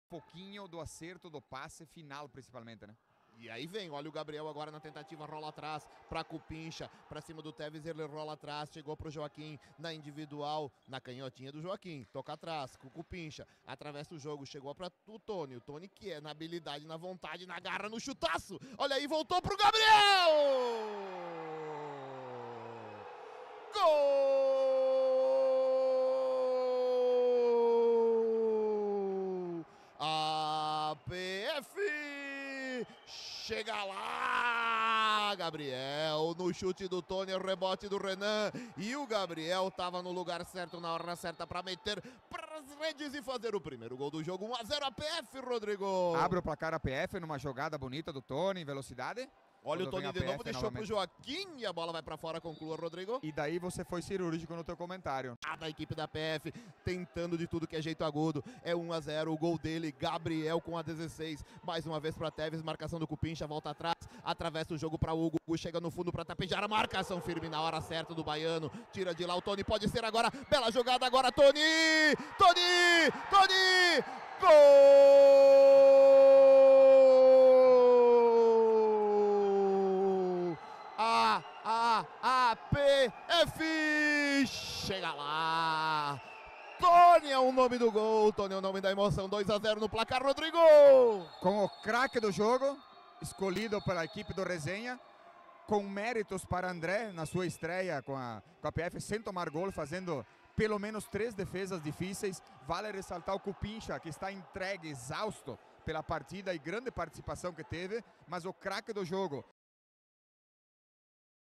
Os gols da vitória da APF 2 X 0 AAGF , Pela Liga gaúcha de futsal 2024.
Gols:
⚽Gabriel
⚽Toni

Créditos das Imagens: @resenharesumodoesporte